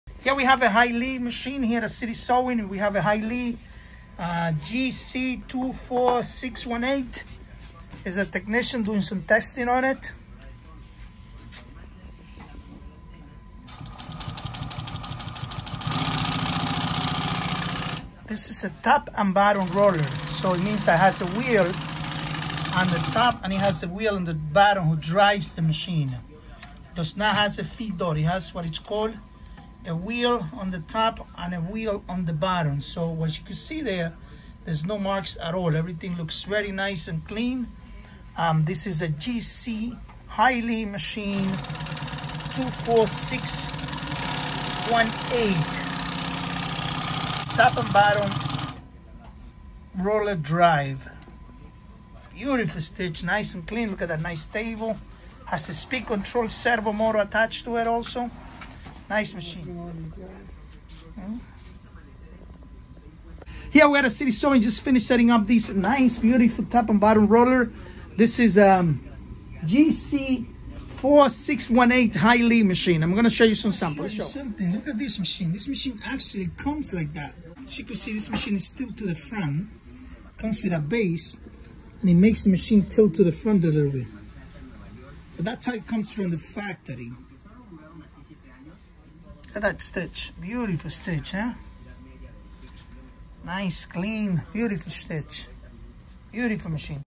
Shoes machine .!!!!
HIGHLEAD GC 24608-1R
Sewing Machine Description MACHINE DESCRIPTION: The Highlead GC24608-1R is a single needle heavy duty compound feed walking foot lockstitch sewing machines which features a right side post-bed design.
The post-bed design is very useful when sewing shaped items or panels, and is used when you need to sew into shaped items which can not be done on a standard flat-bed machine.

The 178mm high slim post bed gives you greater access and control when sewing decorative & functional stitching on items like leather upholstery panels, shoe uppers, car seat upholstery etc.

The machine is smooth running, durable, versatile and its robust construction includes: needle roller bearings for the arm & hook shaft etc., cog driven belt assemblies and a semi-automatic oil lubrication system with sump is fitted which reduces wear & tear considerably.